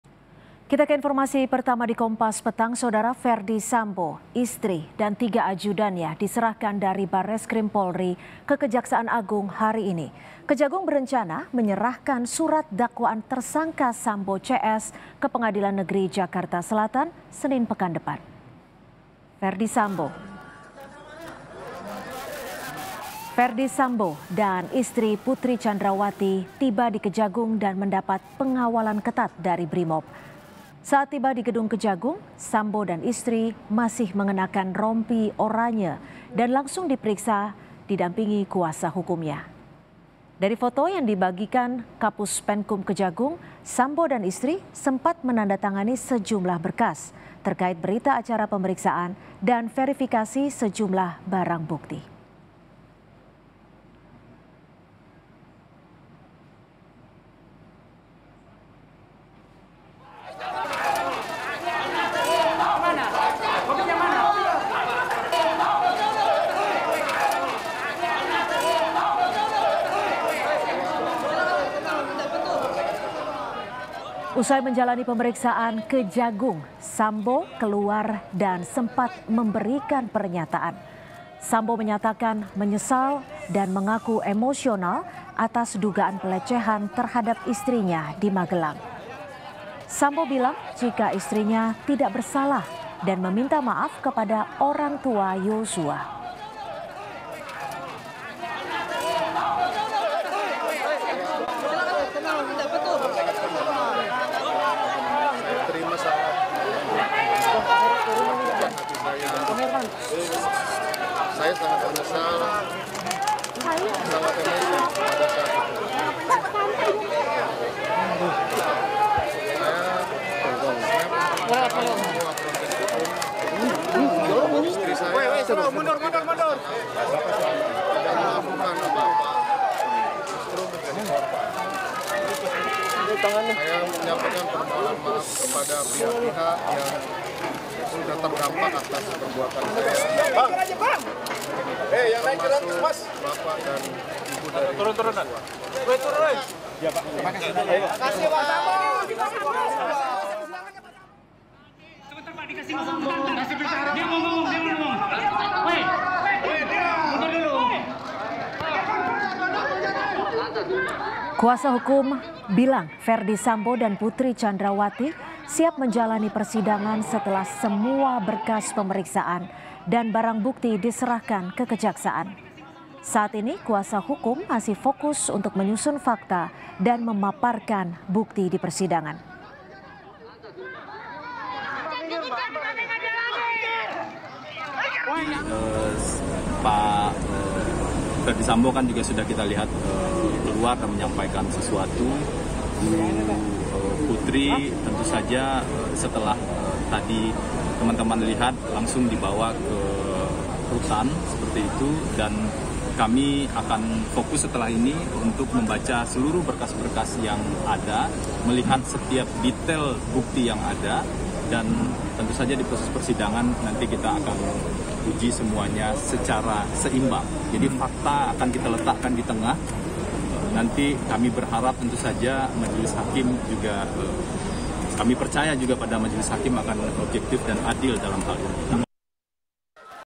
Momen Sambo Nampakkan Diri Usai Jalani Pemeriksaan di Kejagung & Minta Maaf pada Keluarga Yosua!
JAKARTA, KOMPAS.TV - Ferdy Sambo, istri dan tiga ajudannya diserahkan dari Bareskrim Polri ke Kejaksaan Agung hari ini.

Kejagung berencana menyerahkan surat dakwaan tersangka Sambo CS ke Pengadilan Negeri Jakarta Selatan, Senin (10/10) pekan depan.

Ferdy Sambo dan istri, Putri Candrawati tiba di Kejagung dan mendapat pengawalan ketat dari Brimob.

Saat tiba di gedung Kejagung, Sambo dan istri masih mengenakan rompi orange dan langsung diperiksa didampingi kuasa hukumnya.

Baca Juga Pernyataan Ferdy Sambo Setelah Dilimpahkan ke Kejaksaan Agung: Saya Sangat Menyesal! di

Dari foto yang dibagikan kapuspenkum Kejagung, Sambo dan istri sempat menandatangani sejumlah berkas, terkait berita acara pemeriksaan dan verifikasi sejumlah barang bukti.

Usai menjalani pemeriksaan Kejagung, Sambo keluar dan sempat memberikan pernyataan.

Sambo menyatakan menyesal dan mengaku emosional atas dugaan pelecehan terhadap istrinya di Magelang.

Sambo bilang jika istrinya tidak bersalah, dan meminta maaf kepada orang tua Yosua.

Kuasa hukum bilang Ferdy Sambo dan Putri Candrawathi siap menjalani persidangan setelah semua berkas pemeriksaan dan barang bukti diserahkan ke kejaksaan.

Saat ini kuasa hukum masih fokus untuk menyusun fakta dan memaparkan bukti dipersidangan.